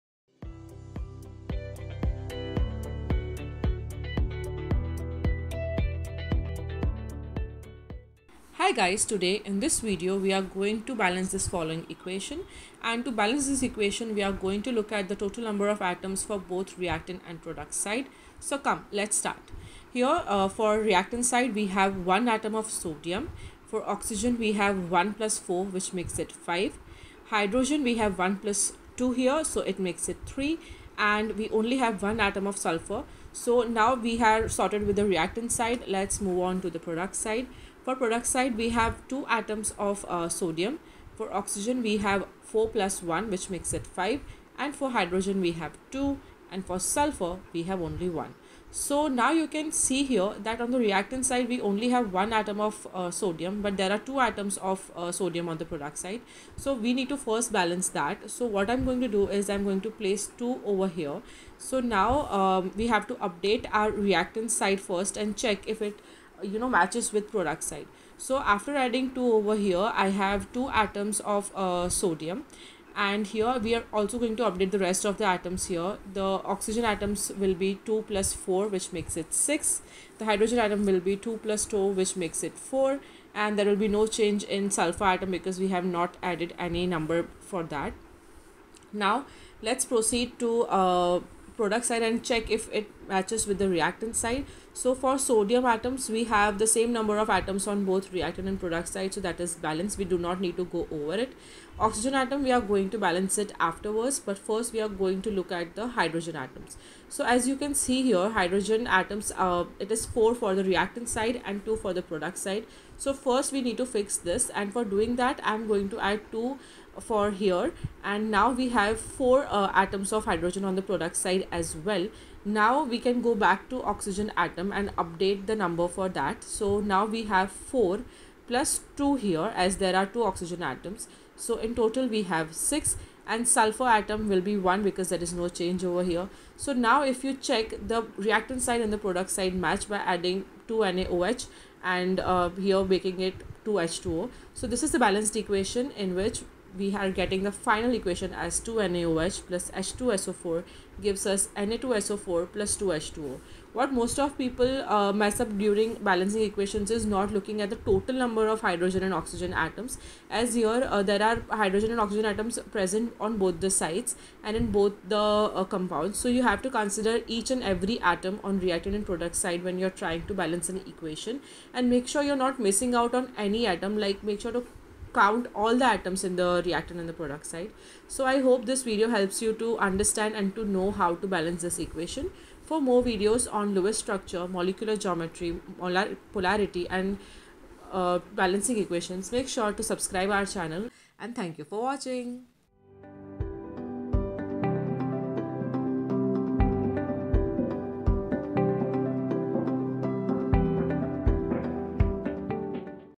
How to balance NaOH + H2SO4 = Na2SO4 + H2O
Hi Guys,

This video will guide you with balancing the equation NaOH + H2SO4 = Na2SO4 + H2O. We will look at the total number of atoms on both the reactant and product side for balancing this equation.

After checking the total number of atoms, we try and balance these atoms on both the side. Watch this video until the end to get the balanced equation and some tips to apply for balancing other equations without any errors.